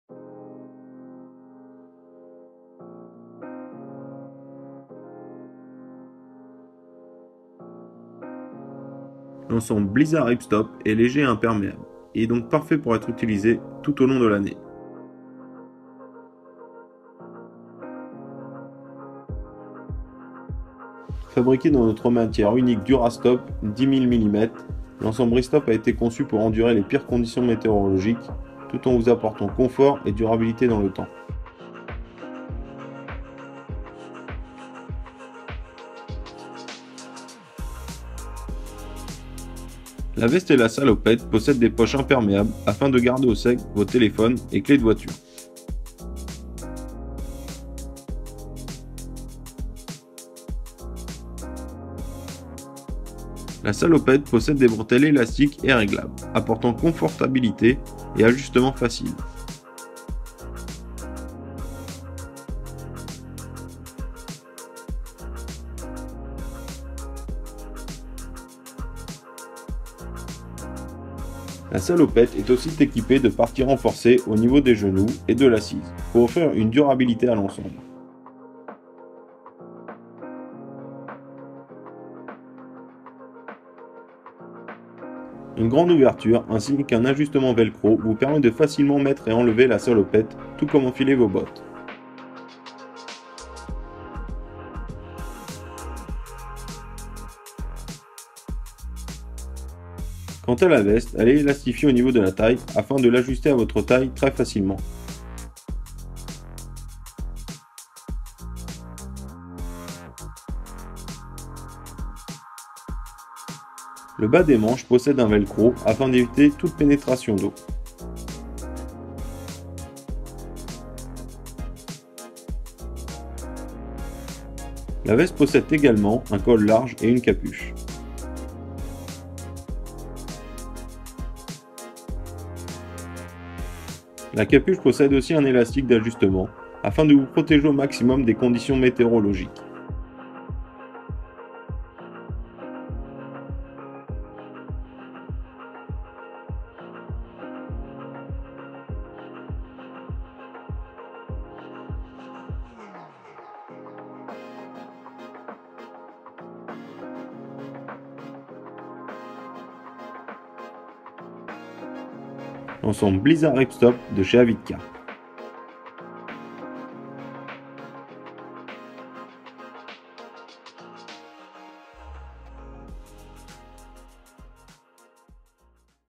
AVID CARP- Blizzard Rip Stop- French
La Blizzard Rip Stop Jacket & Bib & Brace sont les couches imperméables légères ultimes qui peuvent être utilisées toute l'année.

Fabriqué à partir de notre tissu unique Dura-stop de 10 000 mm, le Blizzard Rip Stop Jacket & Bib & Brace a été conçu pour supporter les averses les plus lourdes sans compromettre le confort et la résistance à l'usure.

La veste et le bib and brace sont tous deux munis d'une poche imperméable avec fermeture à glissière sèche pour protéger les éléments comme les téléphones portables et les clés.

Le Bib & Brace est doté de bretelles à ajustement rapide avec base élastiquée, ce qui le rend confortable et facile à ajuster.

Le Bib & Brace est également équipé de panneaux renforcés de genou et de siège pour la longévité supplémentaire sur les zones de salut-usage potentielles.

Des ouvertures de jambes extra larges avec un ajustement velcro et une doublure au toucher doux rendent le Bib & Brace facile à enfiler et à enlever lorsque vous portez des bottes.

La Blizzard Rip Stop Jacket est dotée de bascules élastiques ajustables à la taille pour vous aider à adapter la veste à votre morphologie.

Les poignets ajustables en velcro améliorent également le confort et empêchent l'eau de pénétrer dans les mains mouillées.

La veste est également dotée d'un col haut et d'une capuche à visière escamotable.

La capuche est équipée de boutons-poussoirs élastiques réglables qui peuvent être serrés pour protéger votre tête et votre visage des intempéries.

La Blizzard Rip Stop Jacket & Bavoir de Avid Carp.